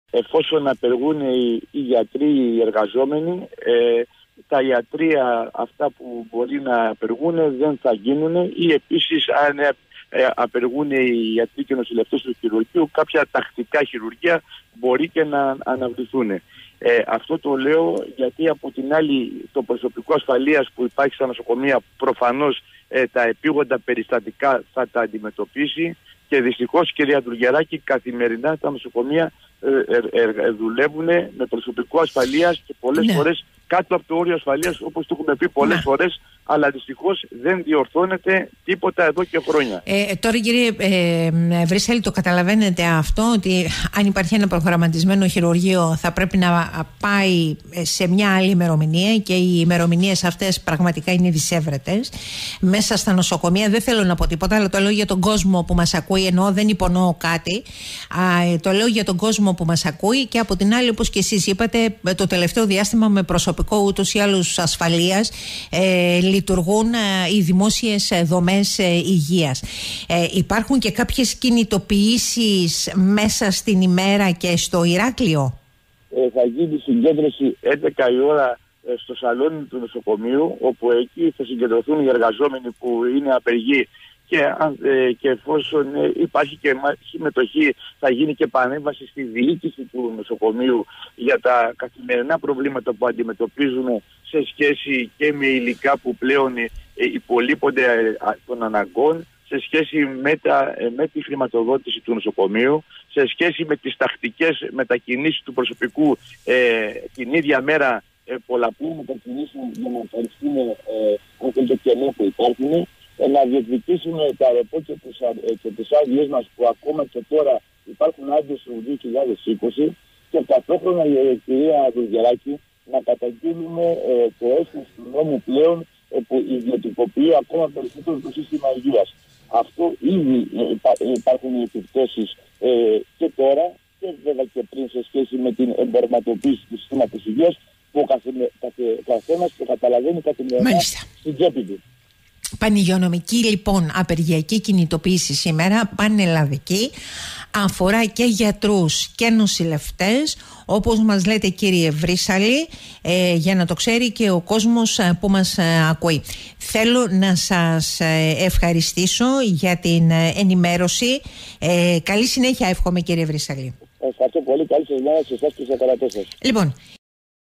Παγυγειονομική απεργία για γιατρούς και νοσηλευτές σήμερα
Σε πρώτο πλάνο οι αποφάσεις κατά της Δημόσιας Υγείας

Πανυγειονομική πανελλαδική απεργία σήμερα Τετάρτη 14 Δεκεμβρίου, ενάντια στο «νέο ΕΣΥ» των ιδιωτικοποιήσεων, των αυξημένων πληρωμών για όλο και λιγότερες υπηρεσίες Υγείας, της μετατροπής των νοσοκομείων σε «επιχειρηματικές μονάδες», όπως καταγγέλλουν οι εργαζόμενοι στο χώρο της υγείας, όπως είπε στον 98.4 ο πρόεδρος του Σωματείου Εργαζομένων στο ΠΑΓΝΗ Δημήτρης Βρύσαλης.